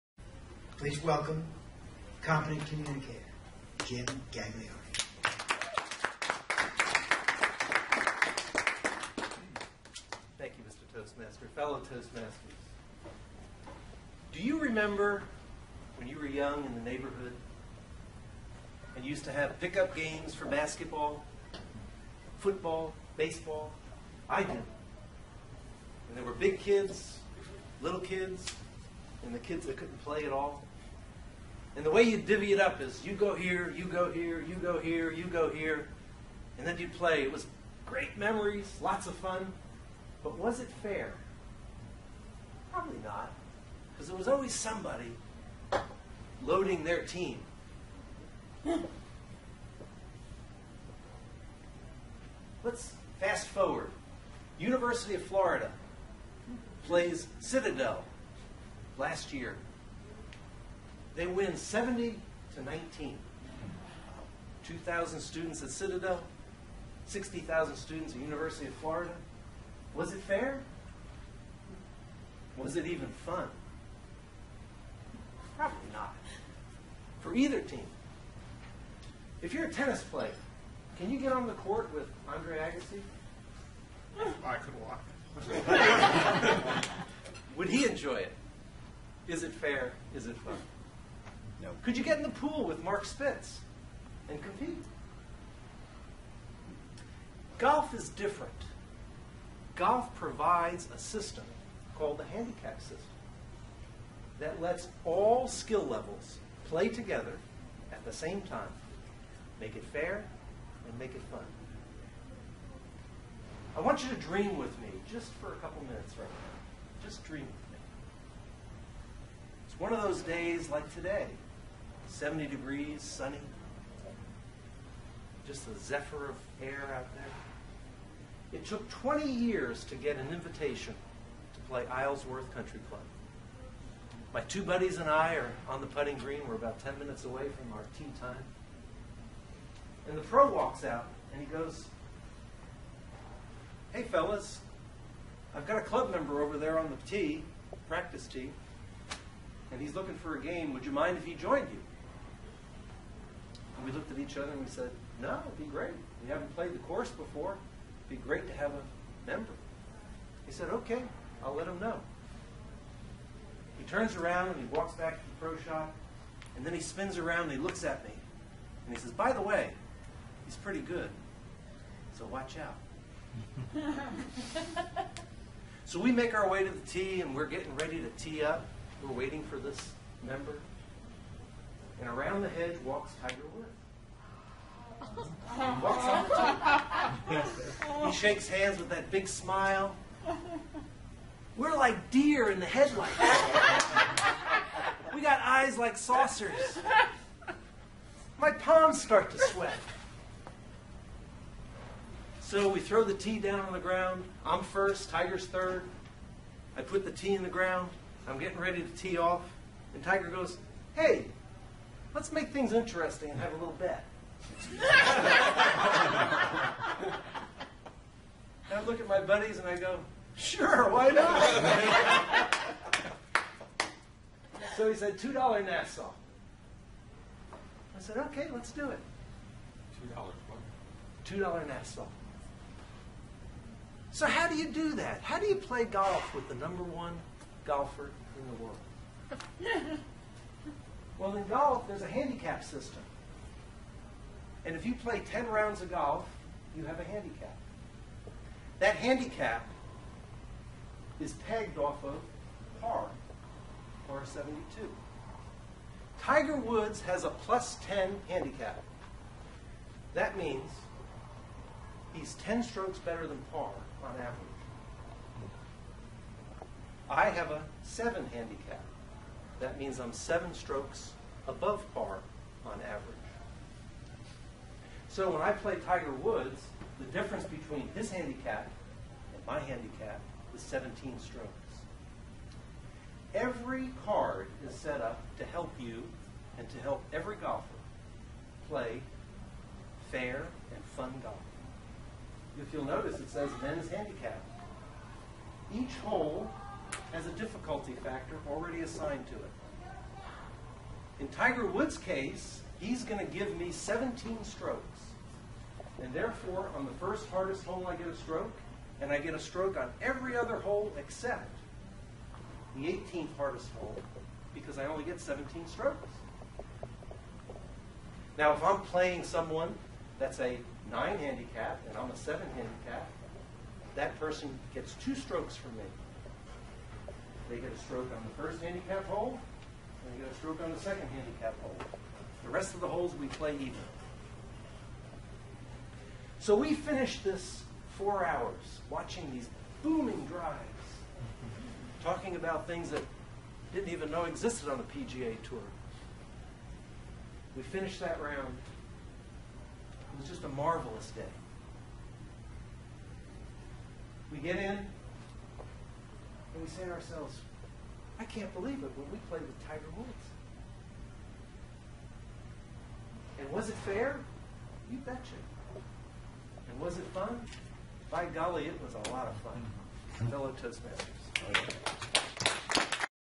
Jim Gagliardi "Chance to Play with a Pro"
This is a great speech about the game of golf and how Jim met Tiger Woods.